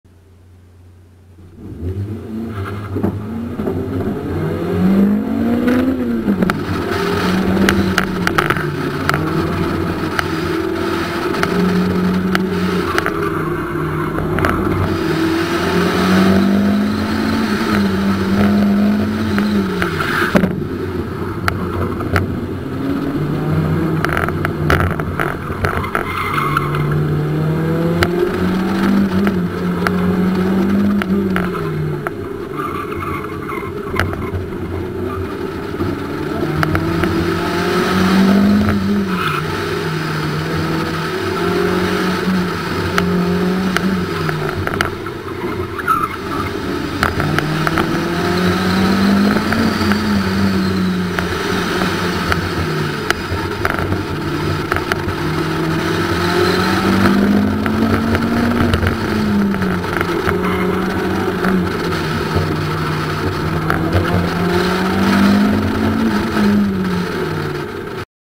Matthew Brower ALSCCA 7 9 20 15 Run 4 61.355s
Fastest run of the day, but still a slow run, 61.355s clean.